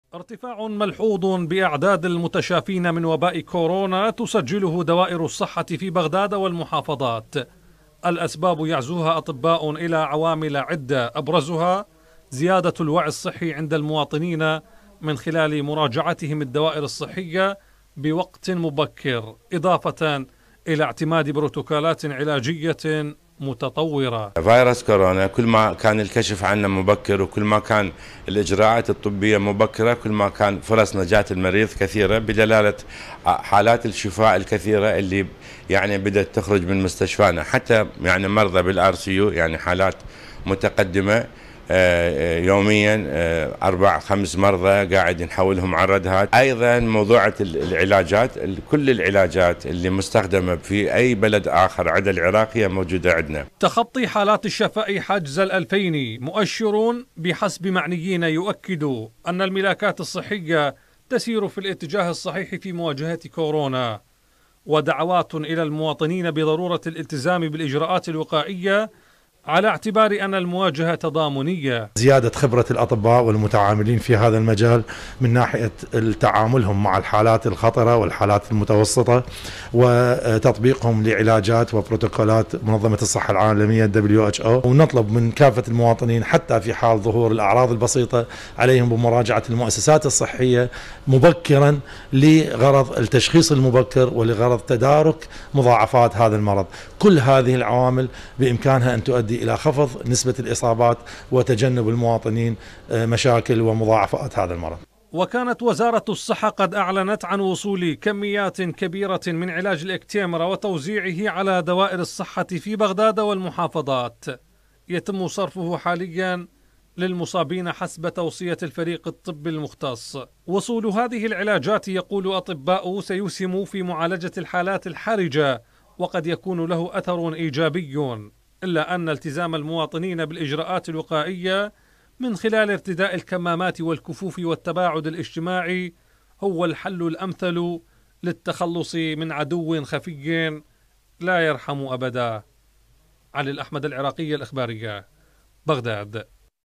معدلات الشفاء من فايروس كورونا تصل مستويات مشجعة
نشرة الثامنة مع فاطمة طالب و حمزة حسين